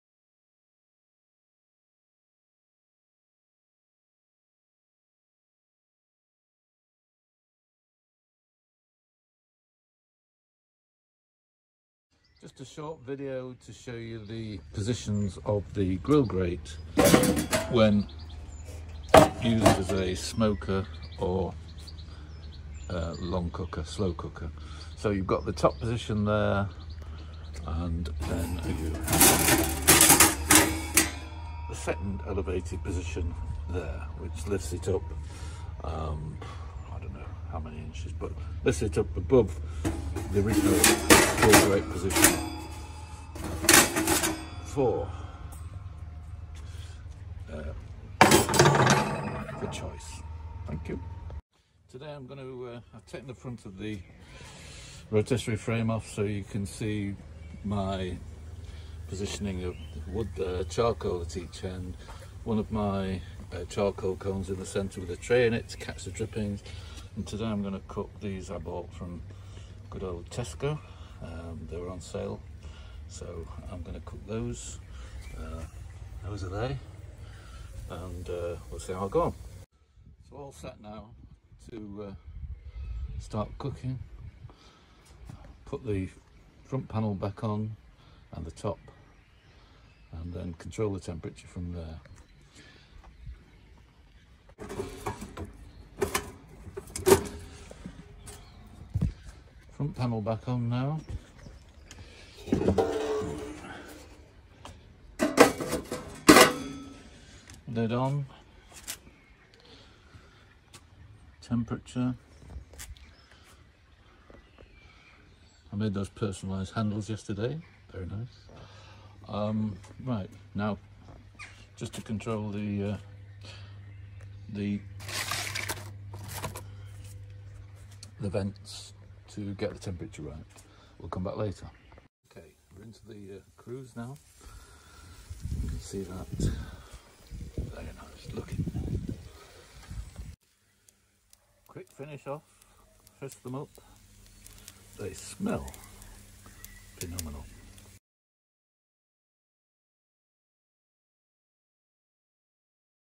Go Anywhere BBQ Flatpack Rotisserie - Riser - Smoker & Slow Cooker
This video demonstrates the grate positions and an example of the rotisserie in action